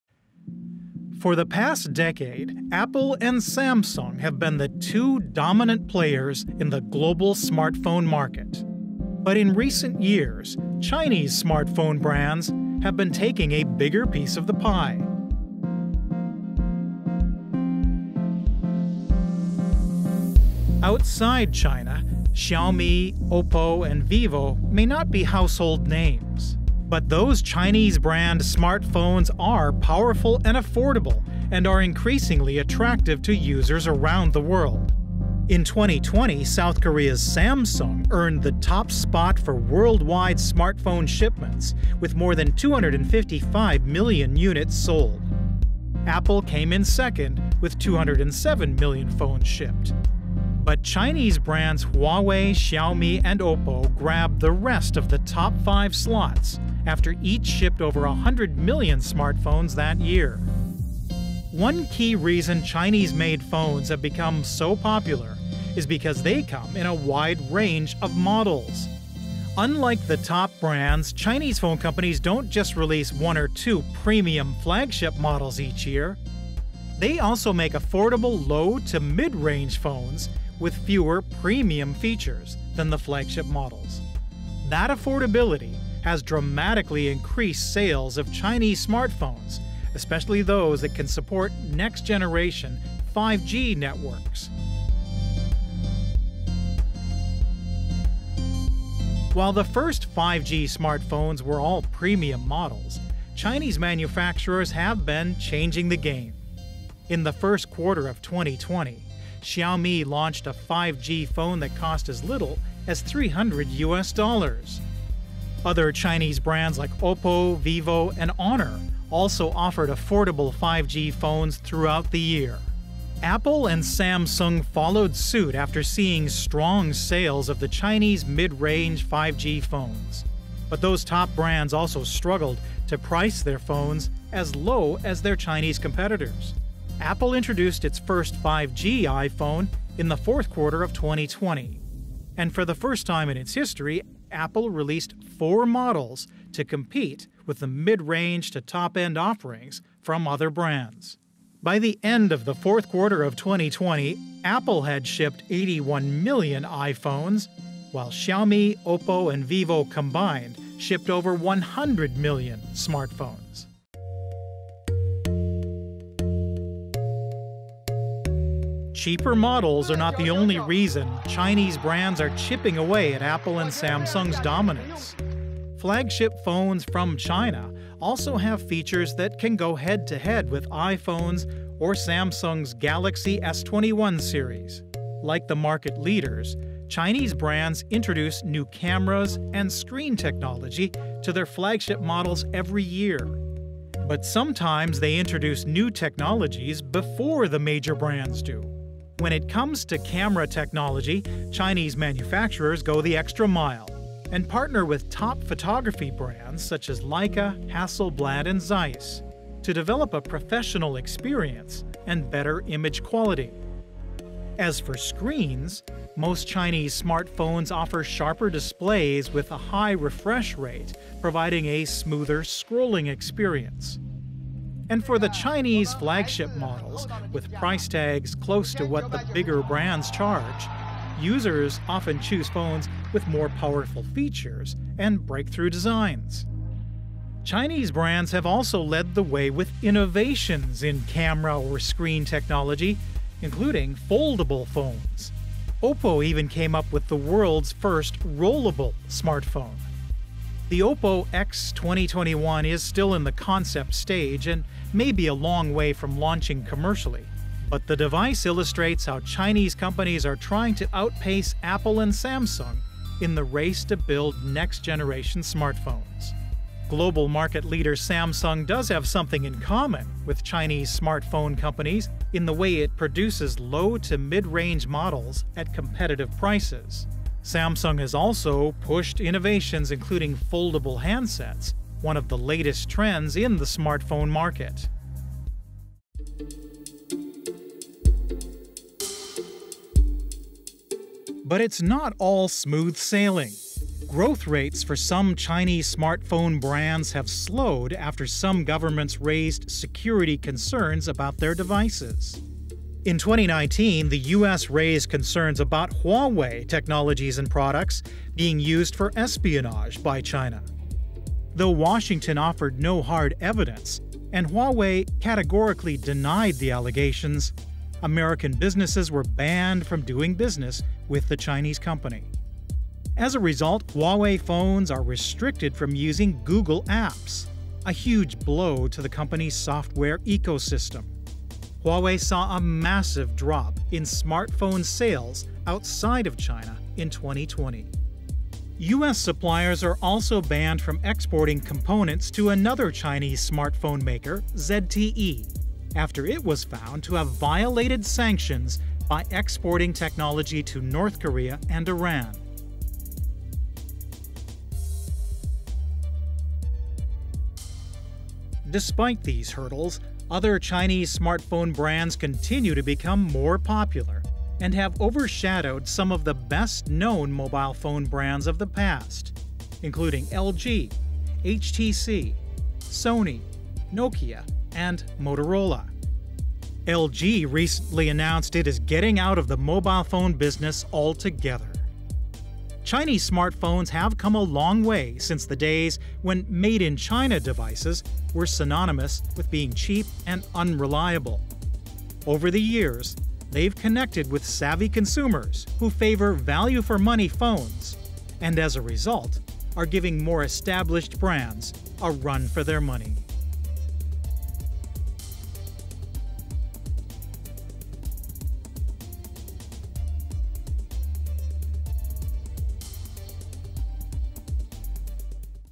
The rise of Chinese smartphones
Chinese smartphones were once thought of as cheaply made, unreliable and not very durable. But in recent years, Chinese smartphone brands have made large inroads to take a bigger share of the global market. The Chinese models have earned a reputation for being powerful yet affordable, and because they support advanced 5G networks across the budget range. Major brands from other countries have struggled to price their phones as low as their Chinese competitors do. As a result, top Chinese smartphone brands such as Xiaomi, Oppo and Vivo are seeing growth in worldwide smartphone shipments to the point that they now provide stiff competition to the likes of Apple and Samsung across the range of smartphone models.